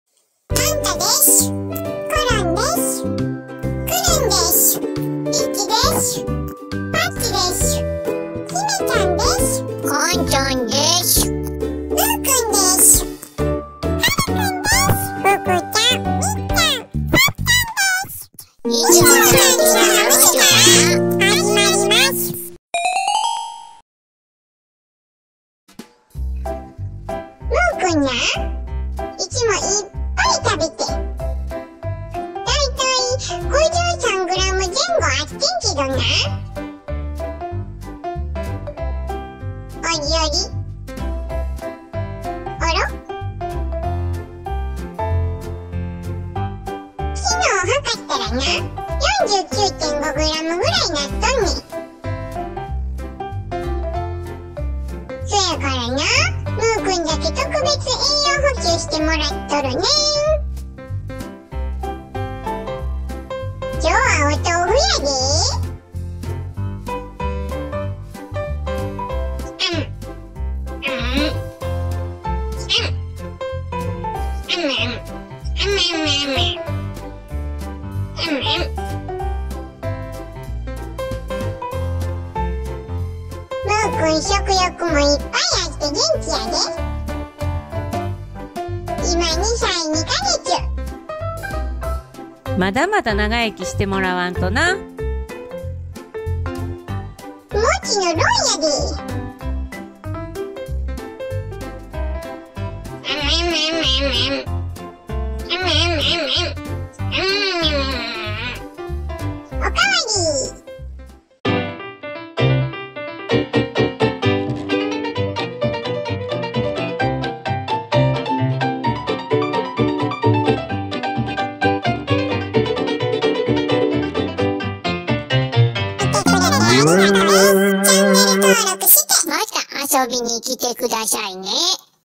【ハムスター】むーくんの体重が減少していた‼️～Mu-kun’s weight was decreasing‼️～の巻
ちょっと心配な出来事がありました。
我が家では定期的にハムちゃんずの体重測定を行っています。
そこで、２歳を過ぎているキャンベルハムスターのむーくんの体重が、少し減ってきていることがわかりました。
エナジーバイトが嫌いな子なので、おとうふなど食べやすい食材で少し食事量を増やしています🐹🍴